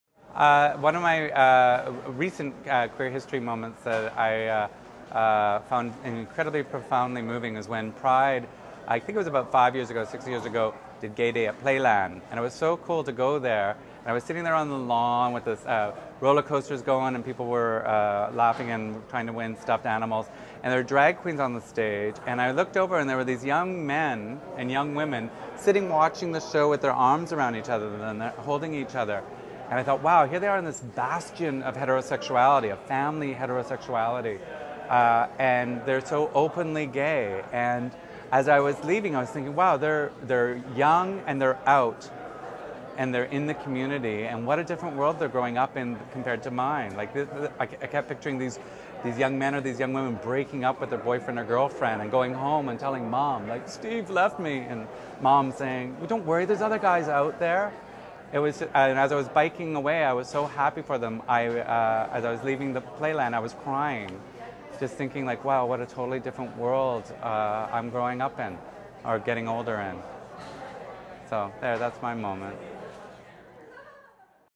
Young men and young women...holding each other.
During the 2009 Vancouver Queer Film Festival the Queer History Project and our Artist-in-Residence local filmmaker Gwen Haworth (She's A Boy I Knew) hosted a creative community-based oral history project—a mobile video tent modeled after City TVs Speakers Corner—called the Speakers Cabaña. Over 75 Festival audience members in shared their own autobiographical stories. These are the questions they were asked - 1. Talk about a memorable queer kiss.
2. What is one of your favorite queer spaces? Why?
3. What was a pivotal moment for you or for your community?